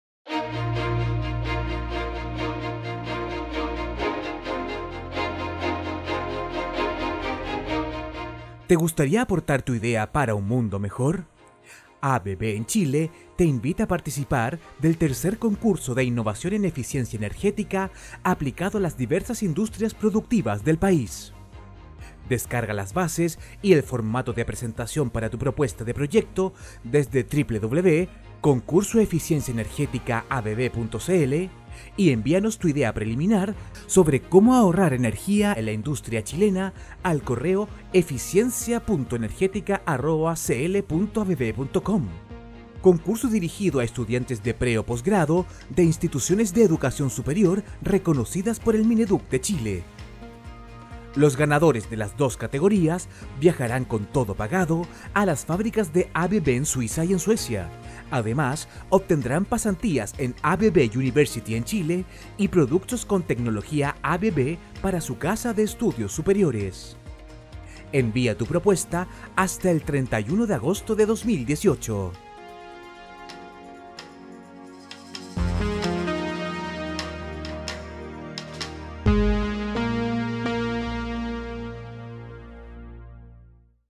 3er Concurso de Innovación en Eficiencia Energética de ABB en Chile (CiEE)
ABB en Chile te invita a participar del 3er Concurso de Innovación en Eficiencia Energética aplicado a las diversas industrias productivas del país. Primera fecha de recepción para “Propuesta de proyectos”: viernes 31 de agosto de 2018 utilizando la cuenta de correo . Para más información